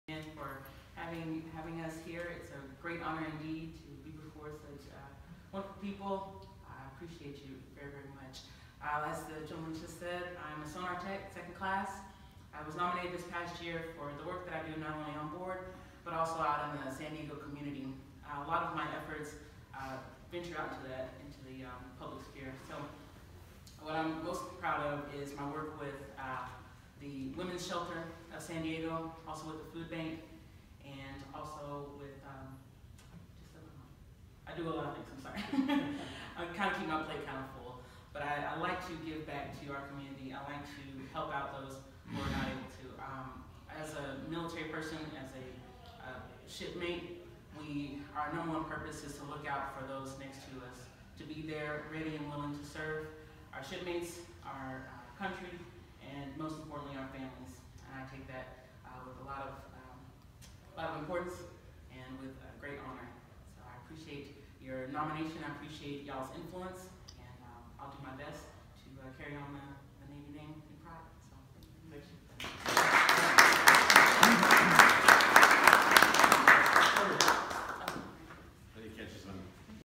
STG2 Jessica Child Sailor of Year 2016
Jessica Child, Sailor of the Year 2016 USS Mobile Bay CG53, gave an acceptance speech at the Navy League of U.S. Greater Palm Springs Council's December 2016 Monthly Meeting.